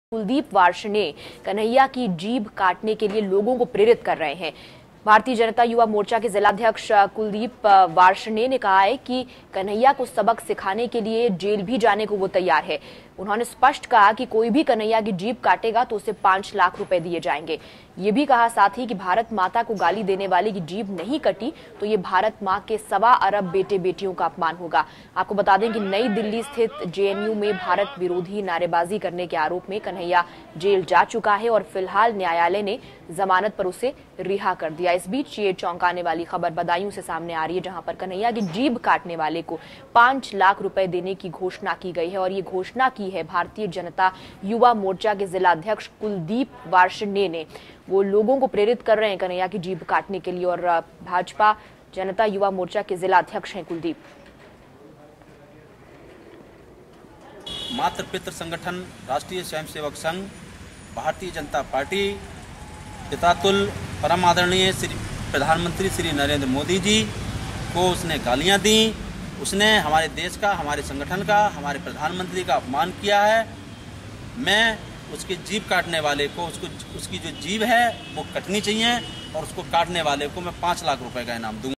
Shocking: 'Will award Rs five lakhs to anyone who chops off Kanhaiya's tongue: BJP leader
In a shocking statement given by a BJP youth leader in Badayun, he has announced a reward of Rs five lakhs on anyone who chops off Kanhaiya's tongue.

For more news, keep watching APN News(TataSky 481)